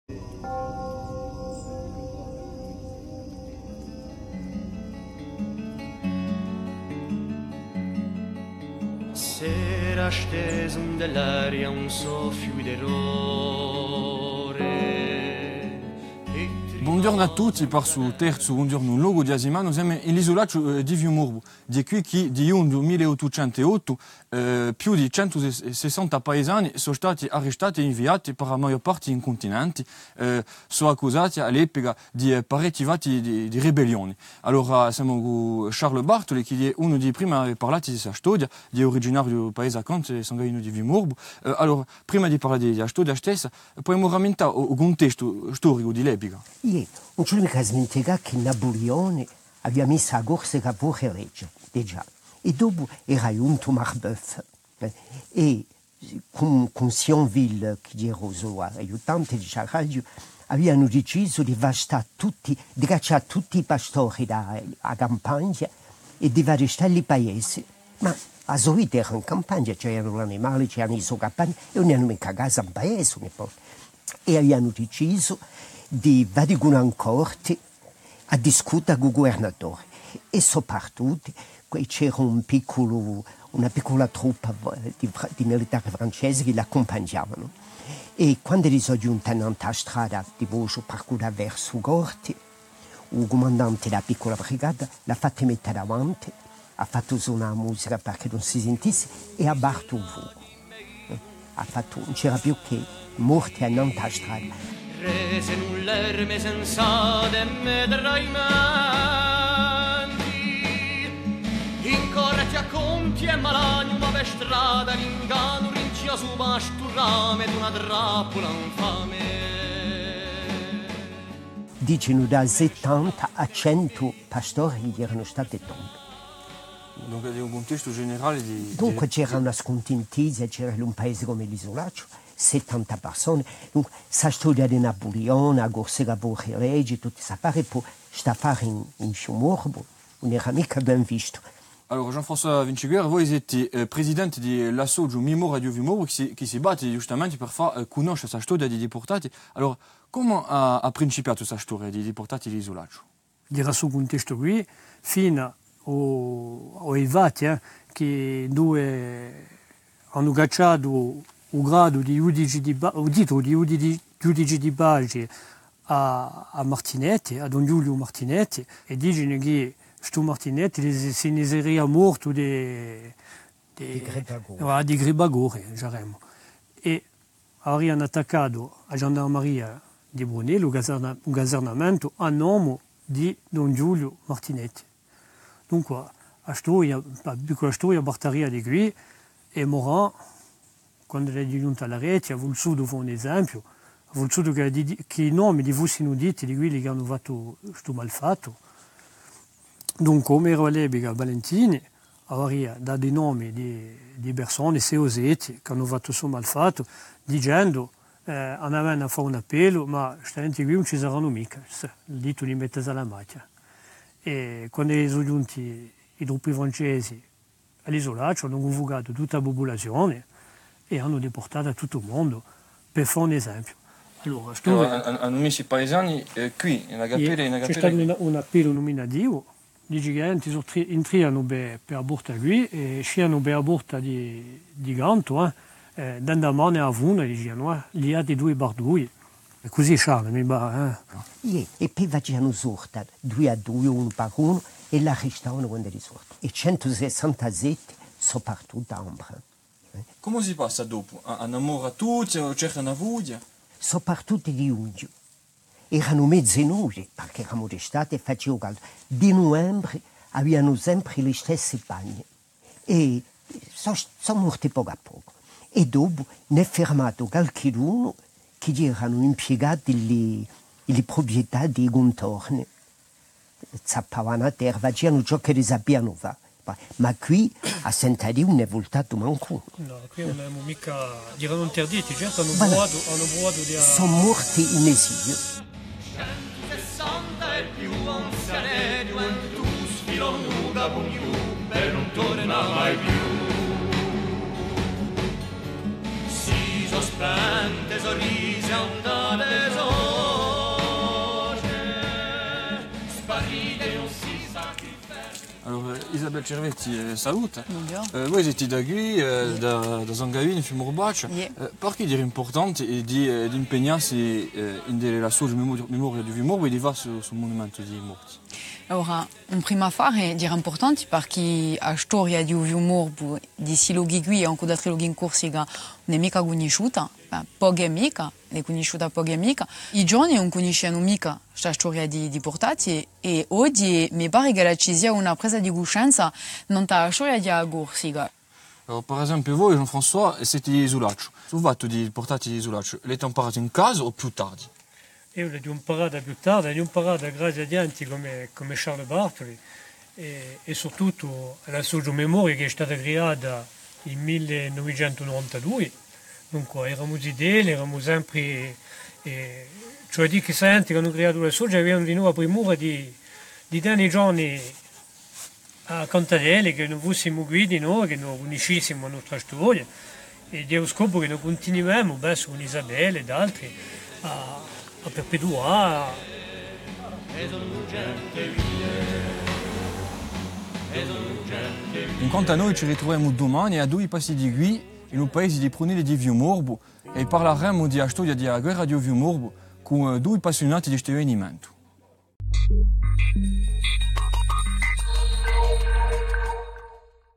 Un ghjornu un locu - Fiumorbu - L'Isulacciu-di-Fiumorbu / 6 Juin 1808
Troisième épisode de "Un ghjornu un locu" sur l'histoire du Fiumorbu. Aujourd'hui direction L'Isulacciu-di-Fiumorbu, ou le 6 Juin 1808, 167 villageois ont été déportés par le Général d'empire Morand.

Equipe - RENUCCI Marc-Antoine / LUCIANI Lionel / CULIOLI Vanessa